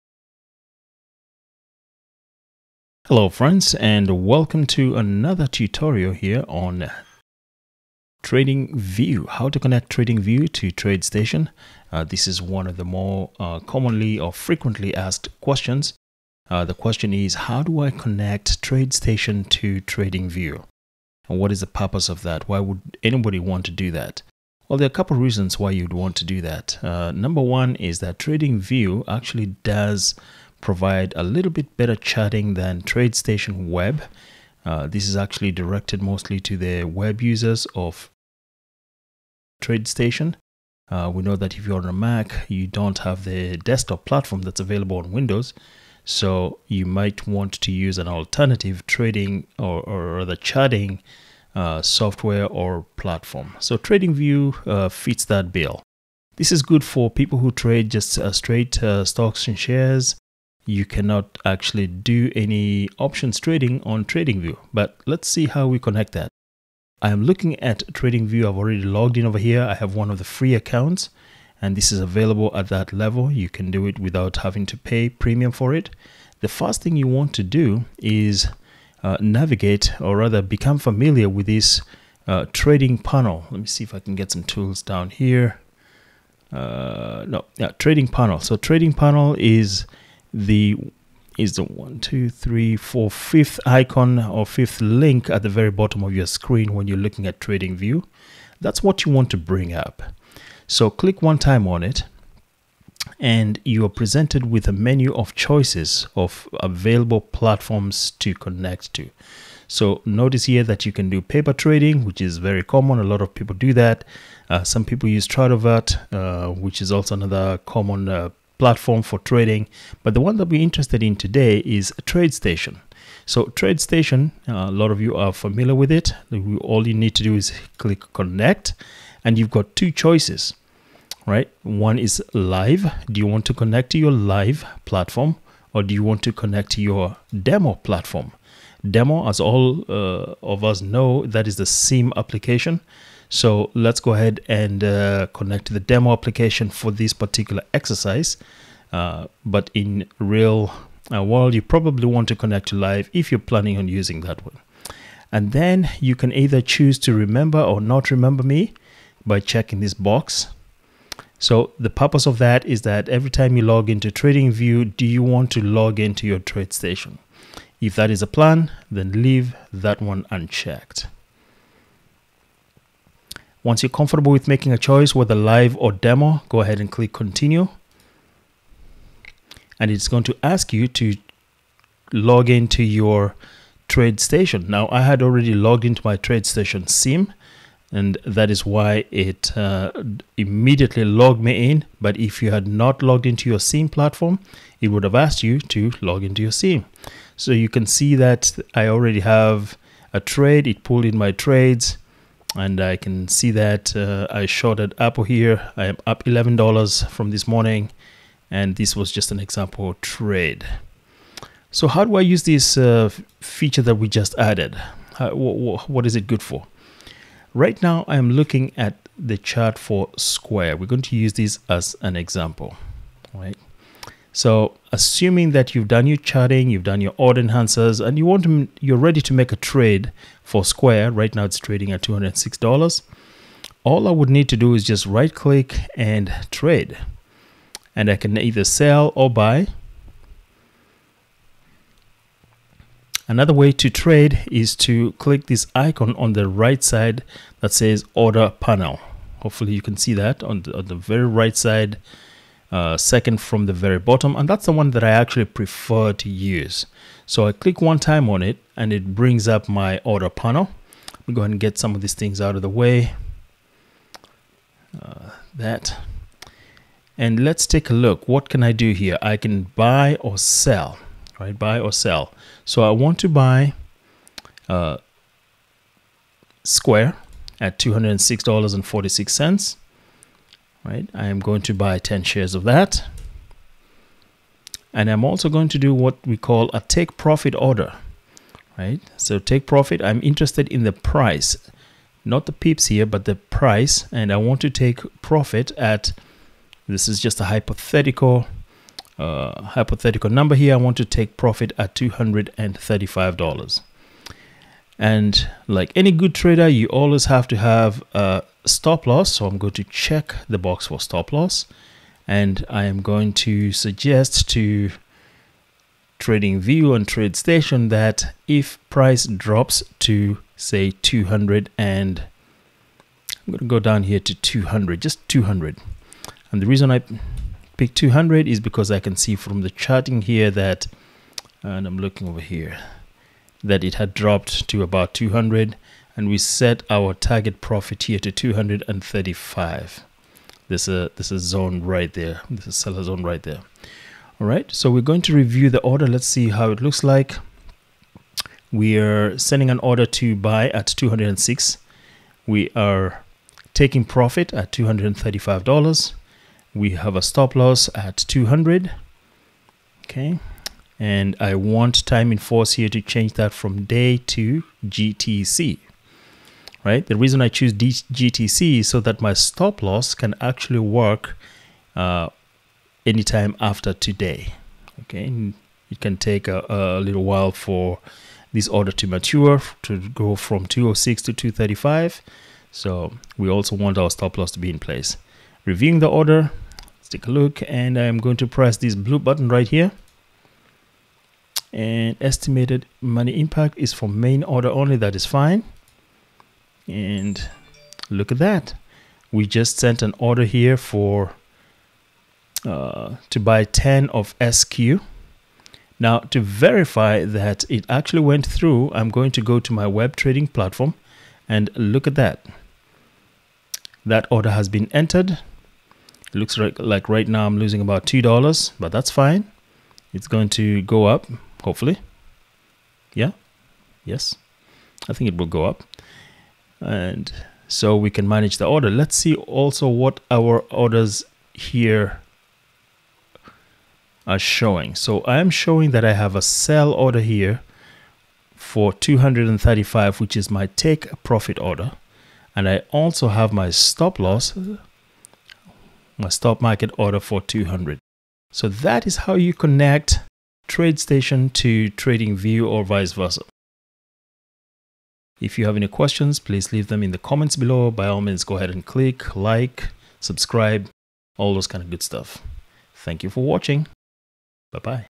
How to connect TradeStation to TradingView
Quick tutorial on how to connect TradeStation to TradingView.
Step 3: Click on the link "Trading Panel" at the bottom of the screen on tradingview
Step 4: Choose whether to connect to Live or Demo (SIM)
Step 5: Enjoy the integration and ability to view or send orders directly to TradeStation

IMPORTANT NOTE:
You can only trade shares on tadingview at this time. No Options trading enabled (I'm looking at you tradingview to enable this in the future ..)